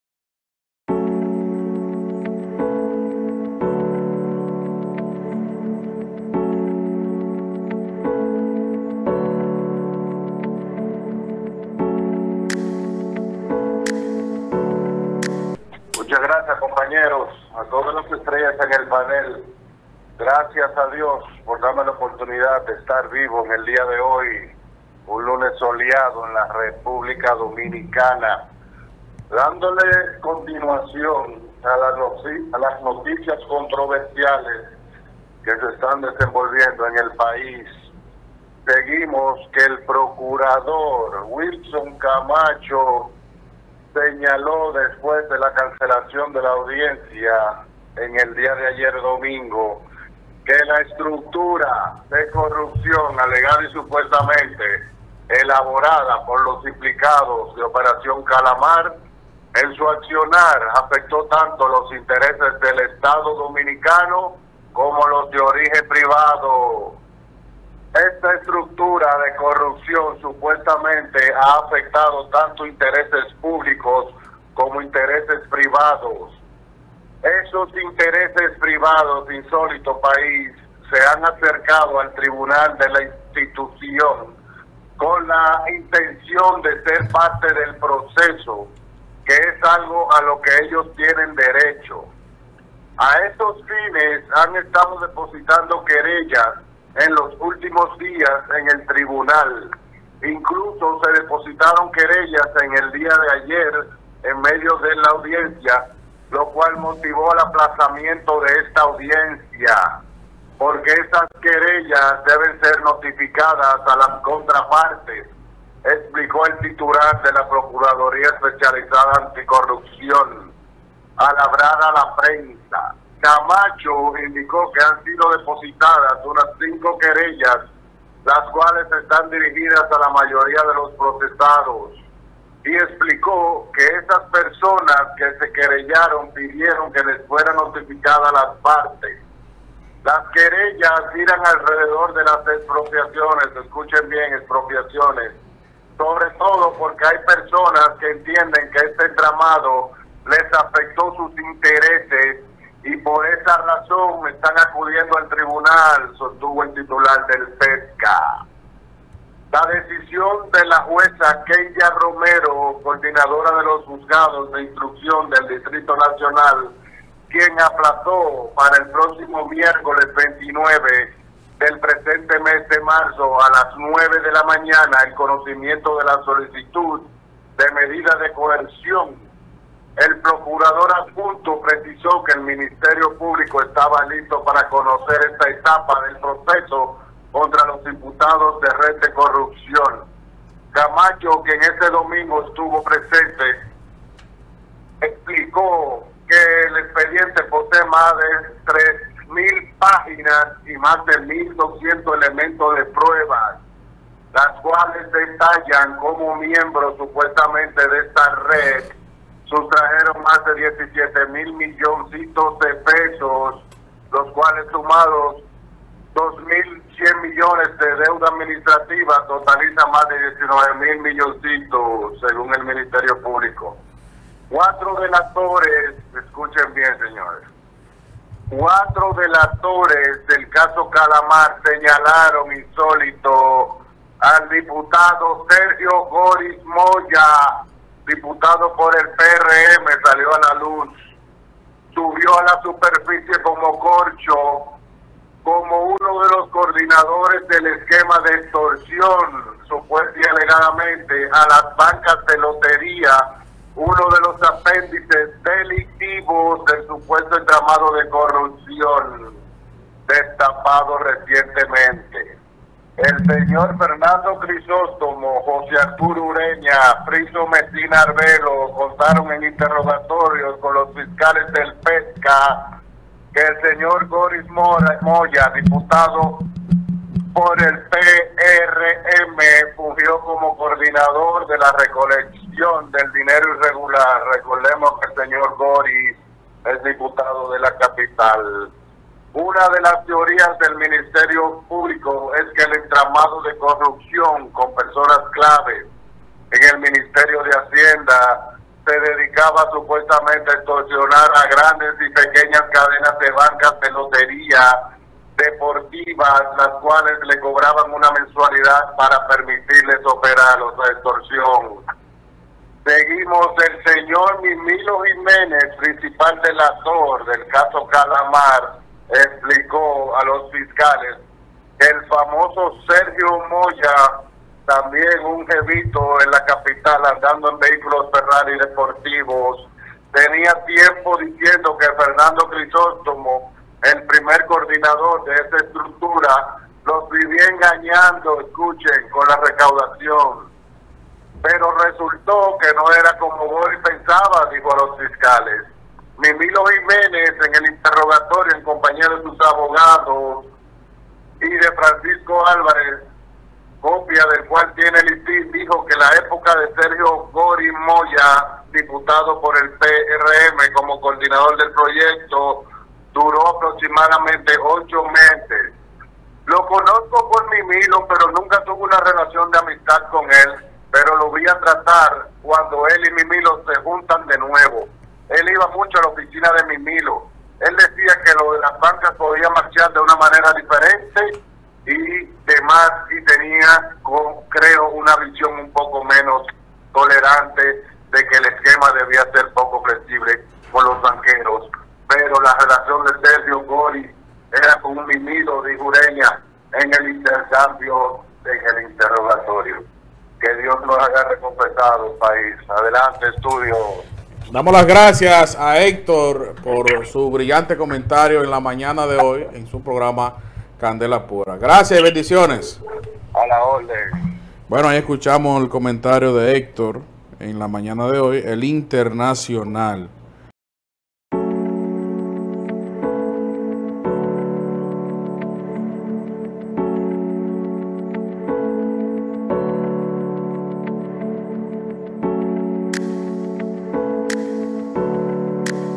De última hora! Revelan diputado del PRM involucrado en mafia de bancas
Cuatro delatores del caso Calamar señalaron al diputado Sergio Moya “Goris” como uno de los coordinadores del esquema de extorsión a las bancas de lotería, uno de los apéndices delictivos del supuesto entramado de corrupción destapado.

Los delatores Ramón Emilio Jiménez Collie, Fernando Crisóstomo, José Arturo Ureña y Frixo Mesina Arvelo contaron en interrogatorios con los fiscales de la Pepca que “Goris” Moya fungió como coordinador de la recolección del dinero irregular. HARR Lic. HéctorArmandoRodríguezRosario analistapolitico.